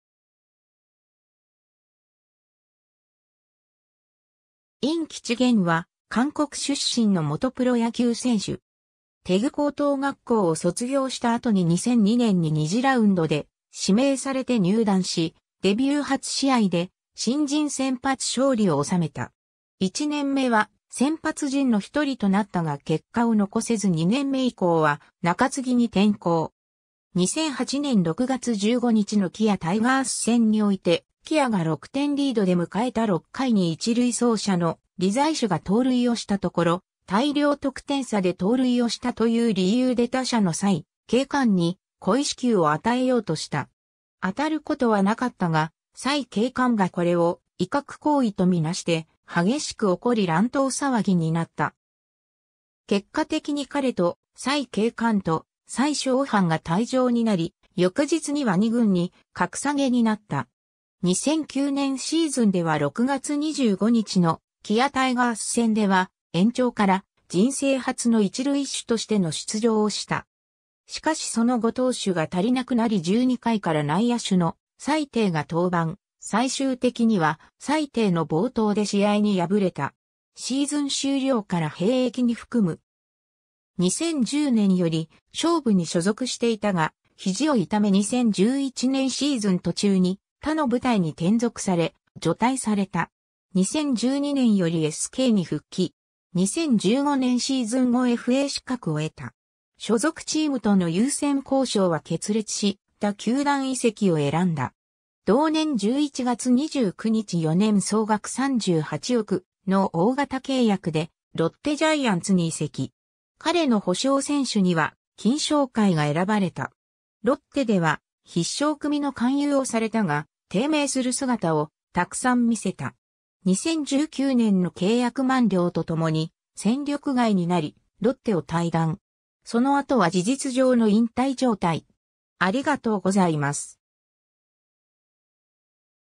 尹吉鉉
ロッテ・ジャイアンツの選手
尹 吉鉉（ユン・キルヒョン、1983年9月23日 - ）は、韓国出身の元プロ野球選手。
 大邱高等学校を卒業した後に2002年に2次ラウンドで指名されて入団し、デビュー初試合で新人先発勝利を収めた。
 1年目は先発陣の一人となったが結果を残せず2年目以降は中継ぎに転向。
 2008年6月15日のKIAタイガース戦において、起亜が6点リードで迎えた6回に1塁走者の李在珠が盗塁をしたところ、「大量得点差で盗塁をした」という理由で打者の崔景煥に故意死球を与えようとした。
当たることはなかったが、崔景煥がこれを威嚇行為とみなして激しく怒り乱闘騒ぎになった。
結果的に彼と崔景煥と蔡鍾範が退場になり、翌日には二軍に格下げになった。
 2009年シーズンでは6月25日の起亜タイガース戦では延長から人生初の一塁手としての出場をした。
しかしその後投手が足りなくなり12回から内野手の崔廷が登板、最終的には崔廷の暴投で試合に敗れた。
シーズン終了から兵役に服務。
 2010年より尚武に所属していたが、ひじを痛め2011年シーズン途中に他の部隊に転属され、除隊された。
 2012年よりSKに復帰。
 2015年シーズン後FA資格を得た。
所属チームとの優先交渉は決裂し、他球団移籍を選んだ。
 同年11月29日4年総額38億（契約金18億ウォン、年俸5億ウォン）の大型契約でロッテ・ジャイアンツに移籍。
彼の補償選手には金承會が選ばれた。
 ロッテでは必勝組の勧誘をされたが、低迷する姿をたくさん見せた。
 2019年の契約満了とともに戦力外になりロッテを退団。
 その後は事実上の引退状態。